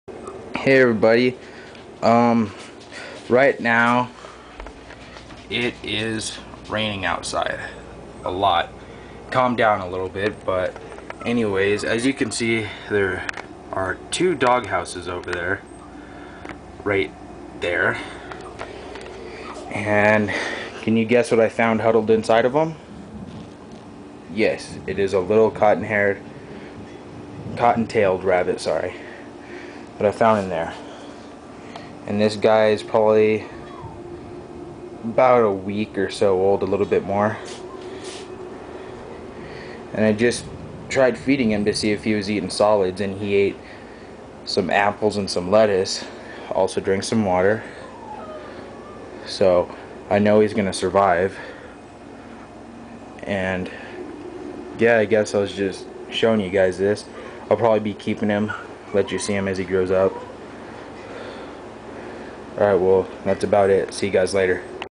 my white tail rabbit
i found this rabbit in my dog house when it was raining t is about a week old.